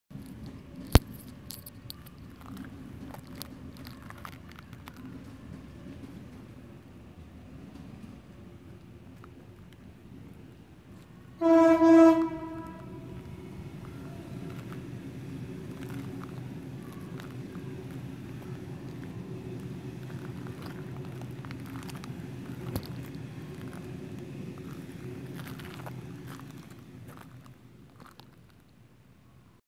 Adz Transport Vlogs - A7 at Redfern (Eveleigh) part 3
This train was heading to Central Sydney Terminal Service (CSTS).